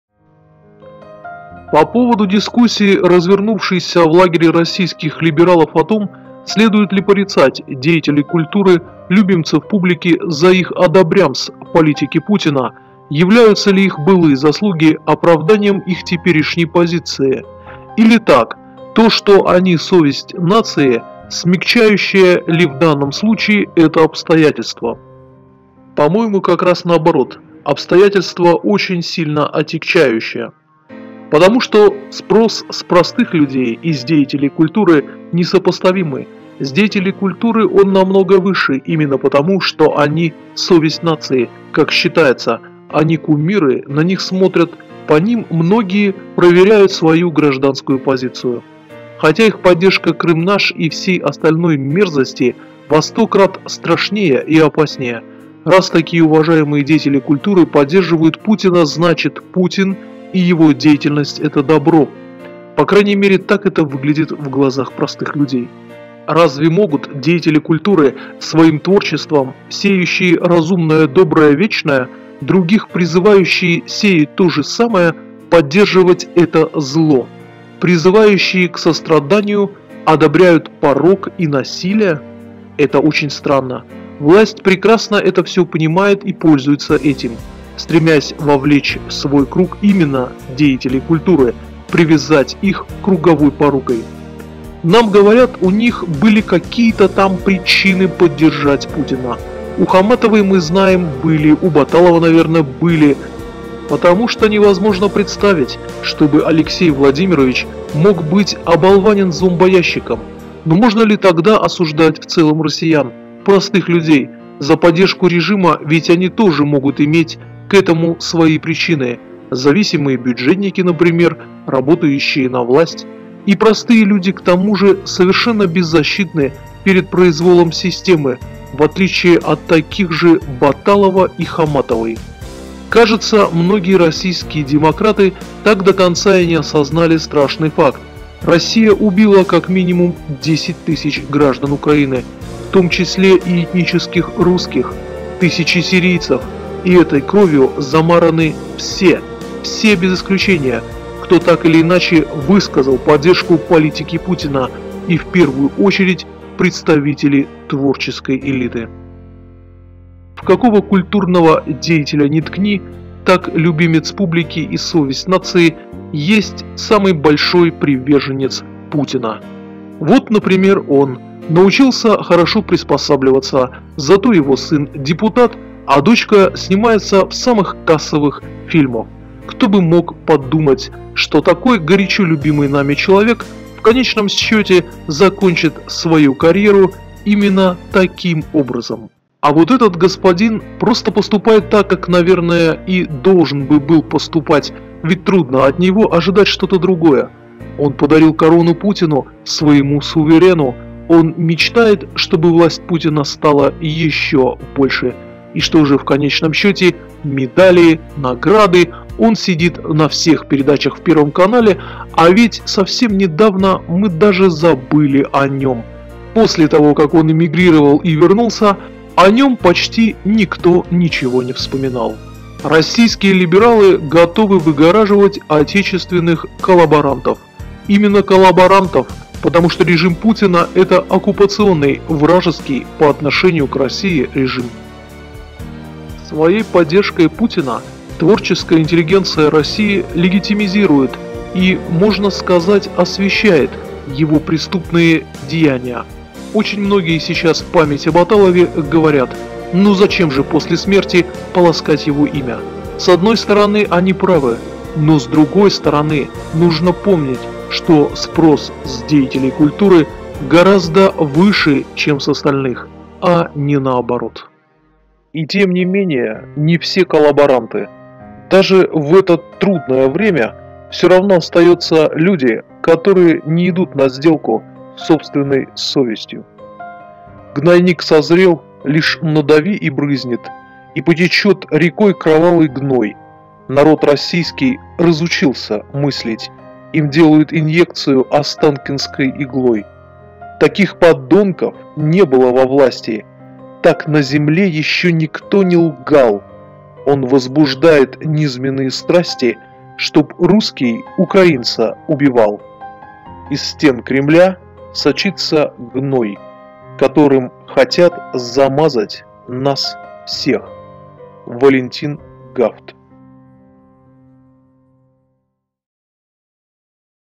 😞🎬 Неуловимые льстители.
Путинский режим поддерживают очень многие. Те, кого было принято считать принципиальными, смелыми, справедливыми. Все они сломались или прогнулись под Путина и его время.